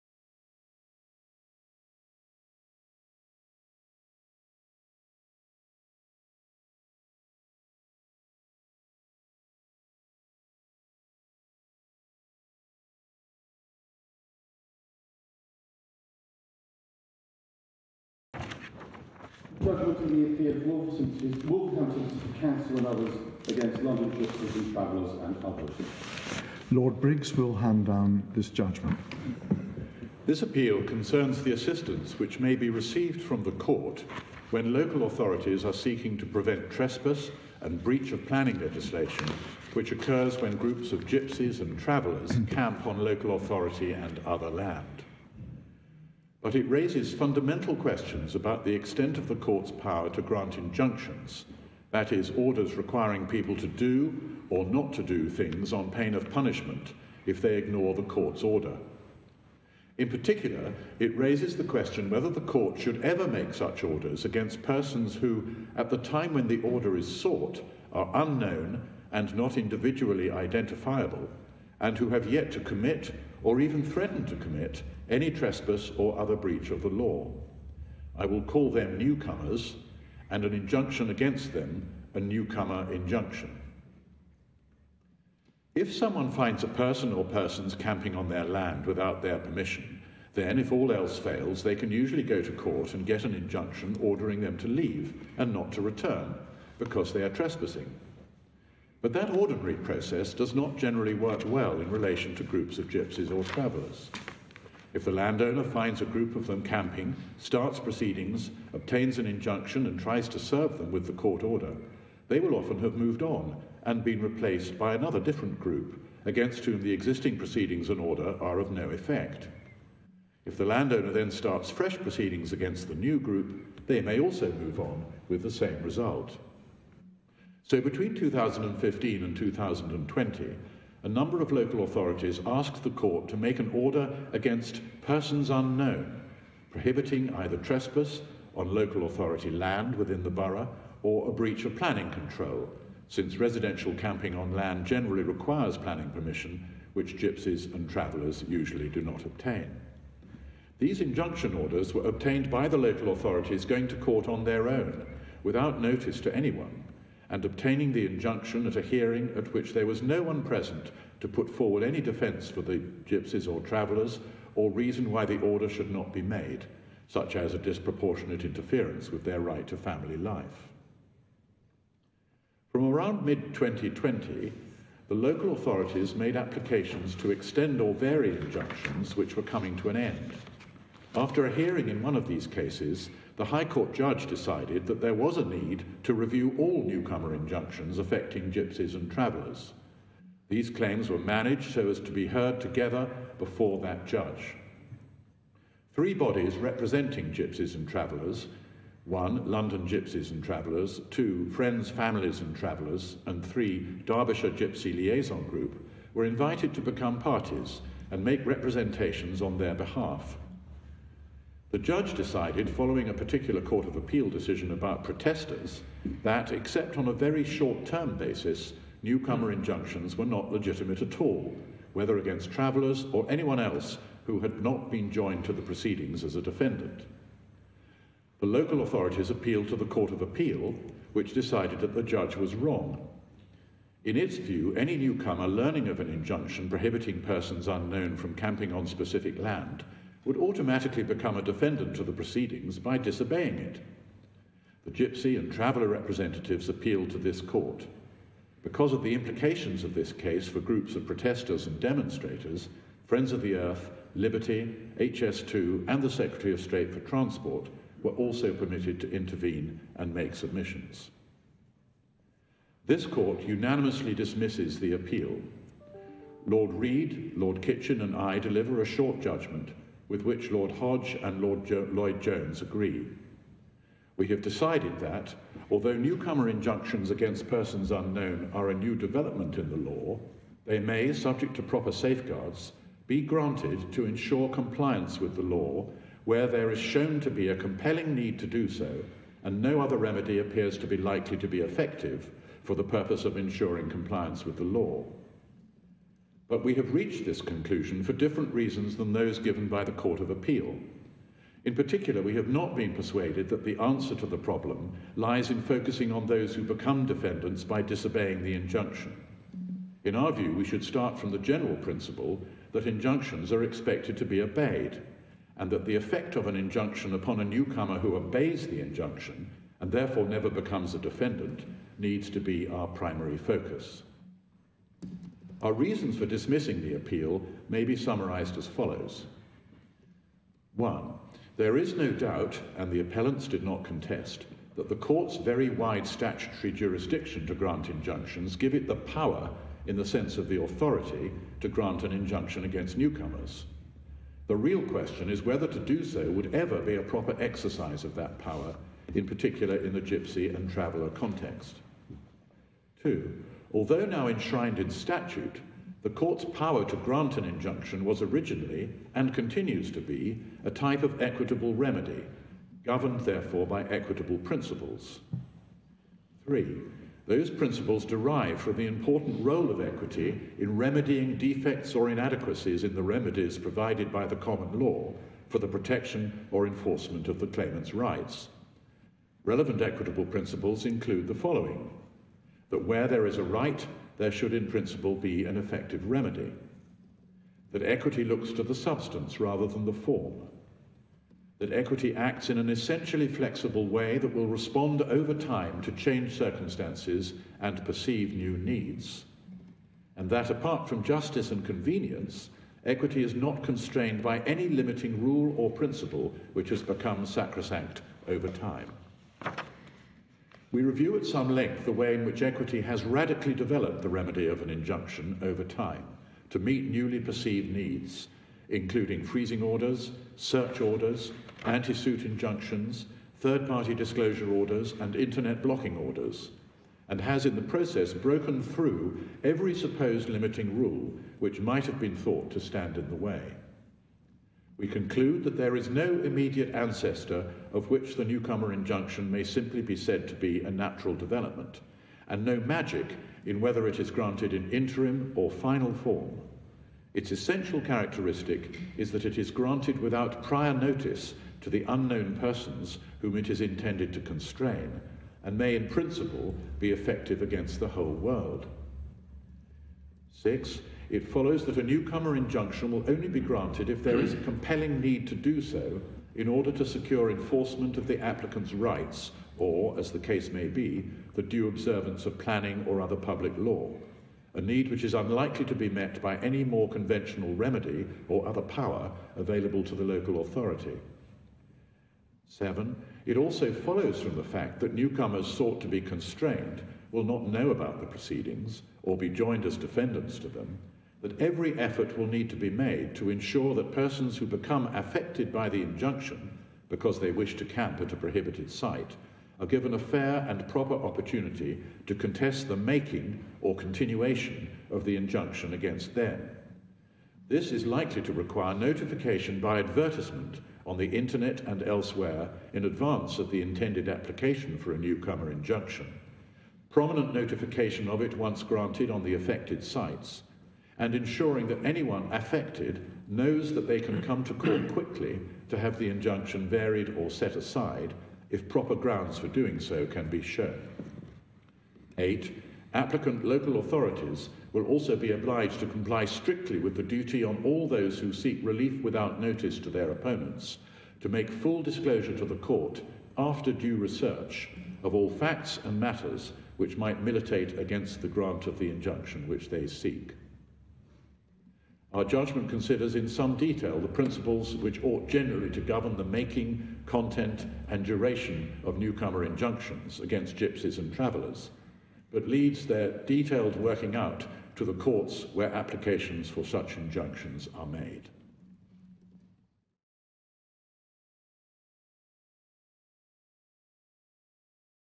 Wolverhampton City Council & others(Respondents) v London Gypsies & Travellers & others (Appellants)
On appeal from [2022] EWCA Civ 13

Between 2015 and 2020, a number of local authorities obtained injunctions designed to prevent Gypsies and Travellers from camping on local authority land without permission. The injunctions were addressed to "persons unknown" because the Gypsies and Travellers who might wish to camp on a particular site could not generally be identified in advance. At the time the injunctions were granted, these unknown persons, or newcomers, had not yet camped, or threatened to camp, on the local authority land without permission, or to commit any other relevant unlawful activity. The local authorities obtained the injunctions without notifying any other party, at hearings where the interests of Gypsies and Travellers were not represented.

From around mid-2020, the local authorities made applications to extend or vary injunctions which were coming to an end. After a hearing in one of these cases, the High Court judge decided that there was a need to review all newcomer injunctions affecting Gypsies and Travellers. He gave the appellants, who represent the interests of Gypsies and Travellers, permission to intervene so that the interests of Gypsies and Travellers could be represented. Following the review hearing, the judge concluded that the court did not have the power to grant newcomer injunctions, except on a short-term, interim basis. He therefore made a series of orders discharging the newcomer injunctions obtained by the local authorities.

The Court of Appeal held that the court had the power to grant newcomer injunctions, and allowed the local authorities' appeal. The appellants now appeal to the Supreme Court.

The issue is:

Does the court have the power to grant "newcomer injunctions", i.e. injunctions against persons who are unknown and unidentified as at the date of the order, and who have not yet performed, or threatened to perform, the acts which the injunction prohibits? If so, on what basis and subject to what safeguards?

The Supreme Court unanimously dismisses the appellants’ appeal. It holds that the court has power to grant newcomer injunctions. However, it should only exercise this power in circumstances where there is a compelling need to protect civil rights or to enforce public law that is not adequately met by any other available remedies. In addition, newcomer injunctions should only be made subject to procedural safeguards designed to protect newcomers’ rights.